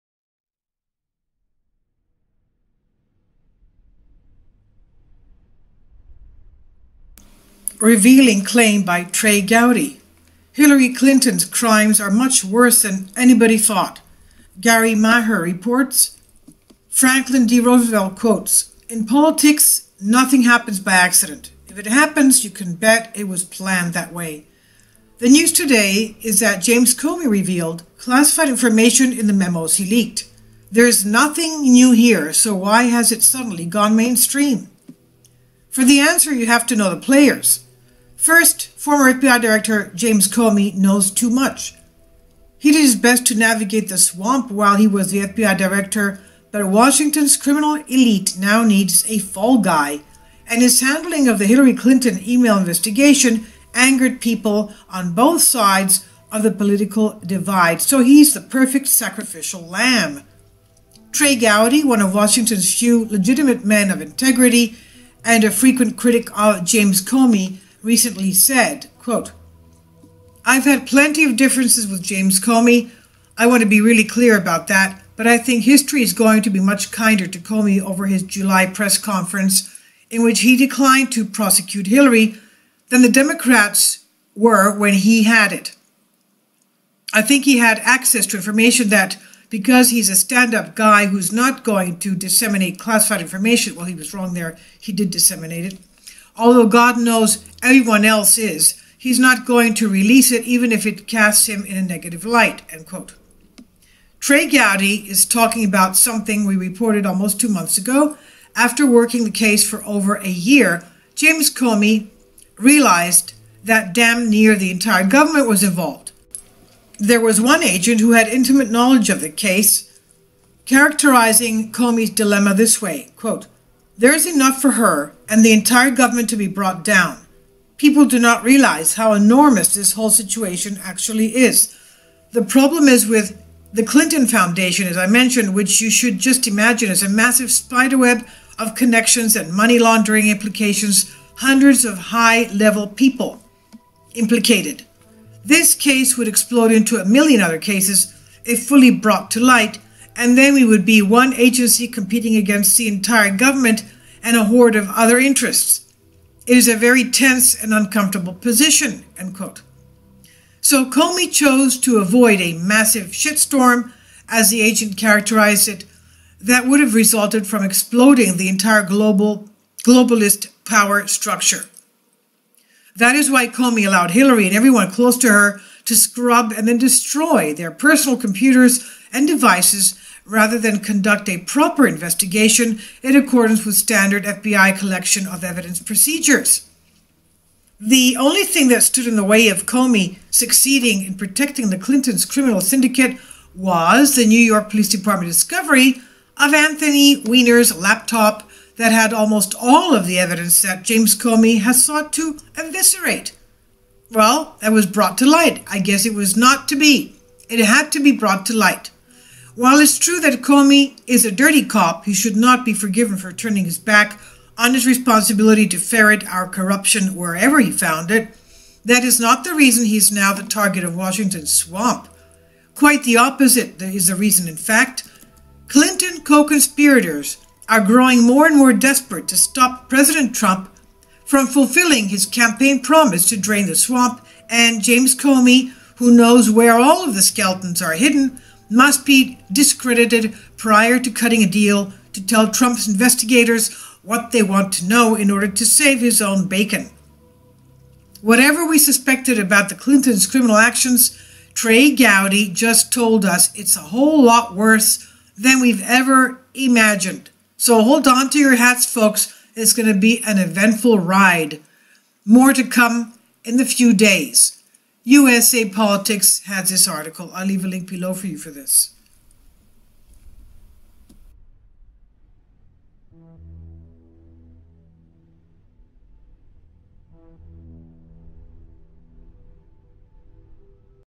EXPOSED: Trey Gowdy - "Hillary Clinton’s Crimes are Much Worse Than Anyone Thought"
Trey Gowdy: Hillary Clinton’s Crimes are Much Worse Than Anyone
Thought.
2) Thumnail image - Trey Gowdy and Hillary crimes worse than believed.jpg